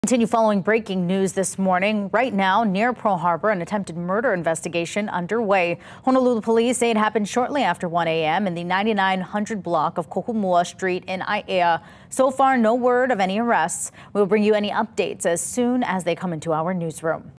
Attempted murder investigation unfolding near Pearl Harbor
An attempted murder investigation is currently underway near Pearl Harbor in Honolulu. The incident occurred shortly after 1 A.M. in the 99-100 block of Kohumua Street in Aiea.